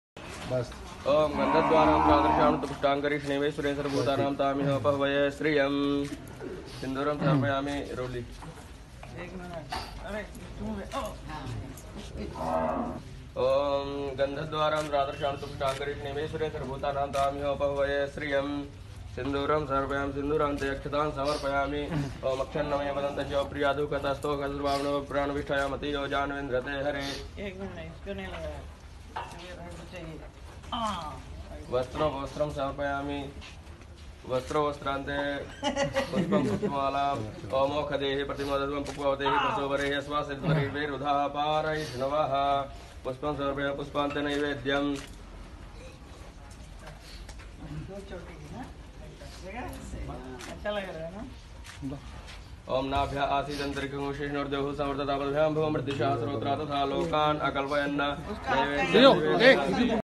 Dream Girl Hema Malini
Hema Malini is an Indian actress, dancer, writer, director, producer, and politician. She is primarily known for her work in Hindi films. Malini made her acting debut in 1963 with the Tamil film Idhu Sathiyam.